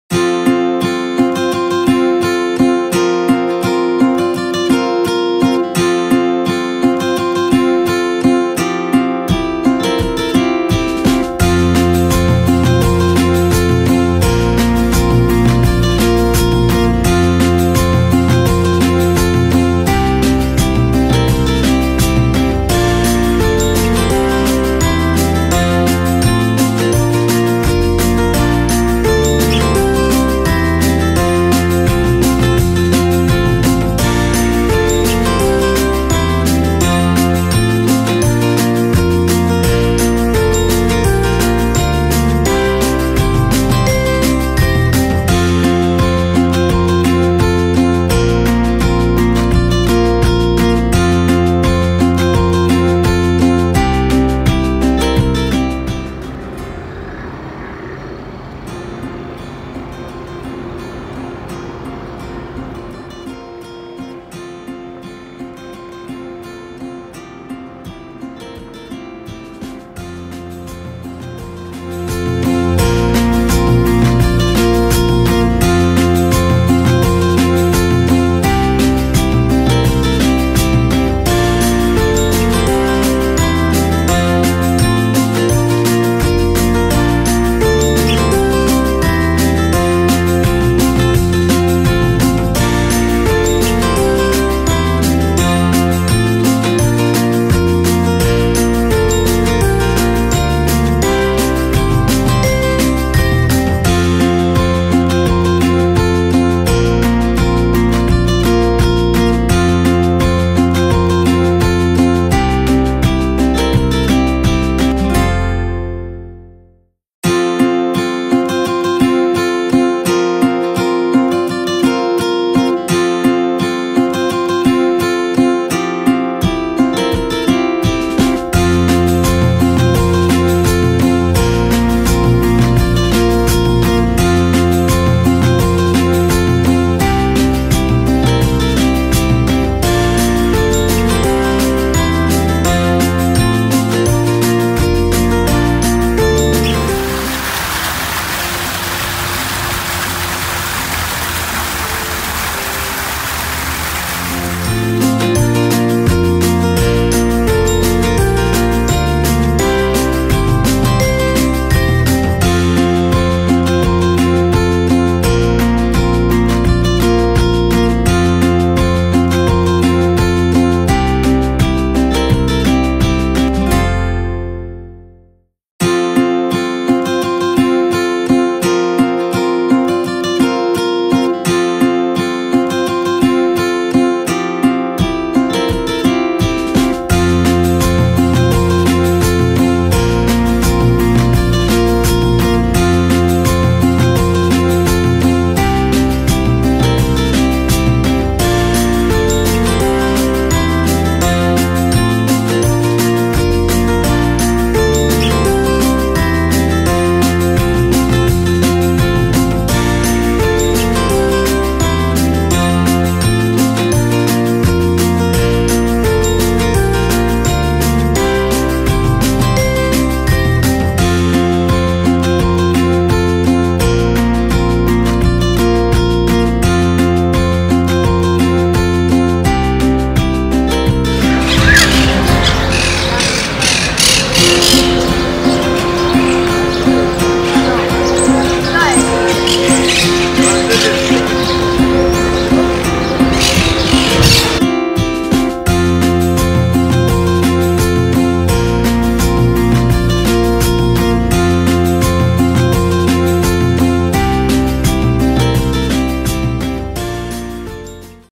Bob does Cartagena
A walk around the Port City of Cartagena Murcia